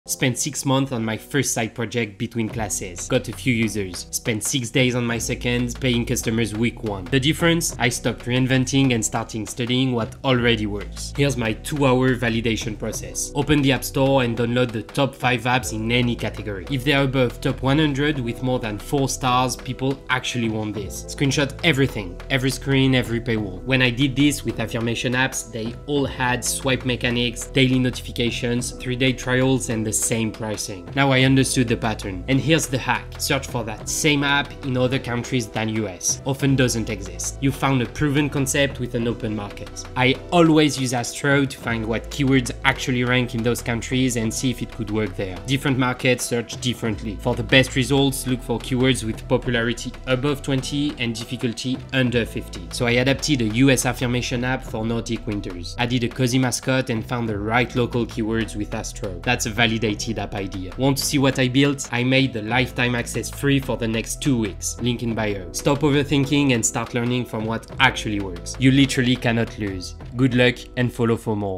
How I validate app ideas in 2 hours (and why your idea doesn't matter)
Spent 6 months on my first project. Few users. Spent 6 days on my second. Paying customers week one. The difference? I stopped reinventing.